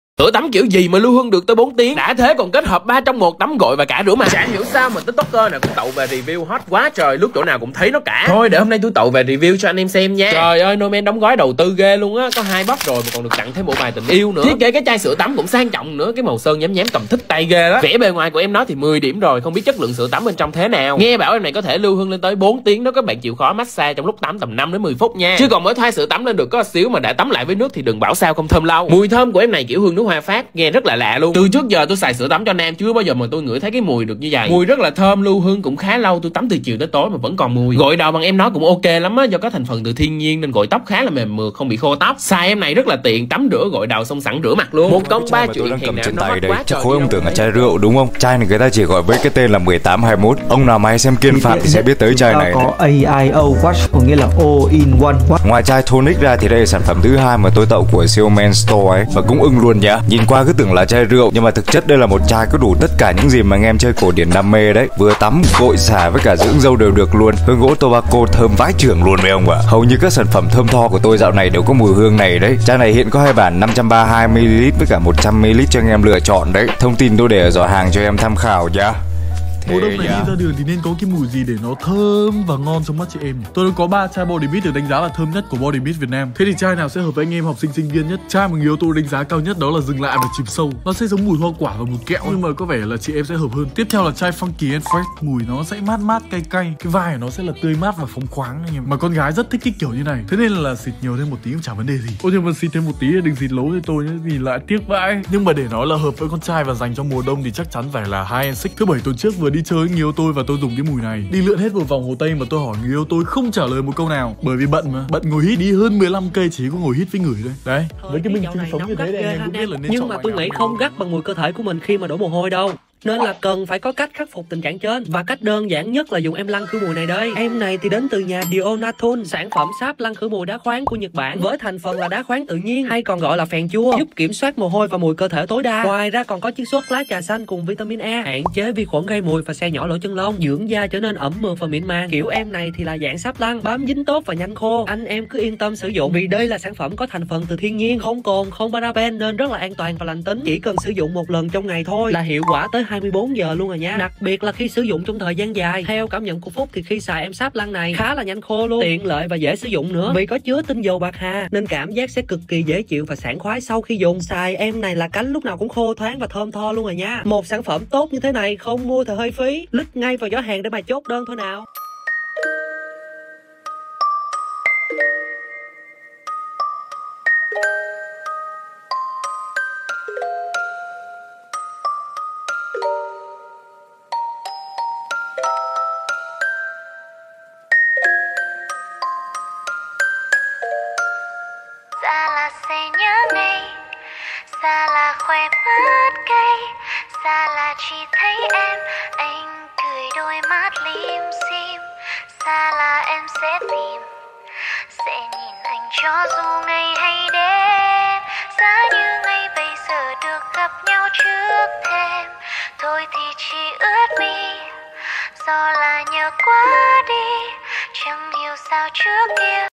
Tổng hợp sản phẩm THƠM CƠ THỂ cho nam | Lăn khử mùi trị hôi nách
Tổng hợp sản phẩm THƠM CƠ THỂ cho nam | Năn khử mùi trị hôi nách
ĐÂY LÀ NHỮNG CÁCH THƠM CƠ THỂ NAM GIỚI MÌNH CRE TRÊN TIKTOK.
Nếu anh em cũng muốn " THƠM THO " như vậy thì sản phẩm tham khảo bên dưới, hãy click vào link:

0:20
3. Sữa tắm, gội, rửa 3 trong 1 NerMan

1:00
2. Sữa tắm, gội, xả, dưỡng Man Made Wash

1:30
4. Bodymist Nam giới lưu hương 6 tiếng

2:10
1. Lăn khử mùi Nam đá khoáng

3:00
5. Sữa tắm nam body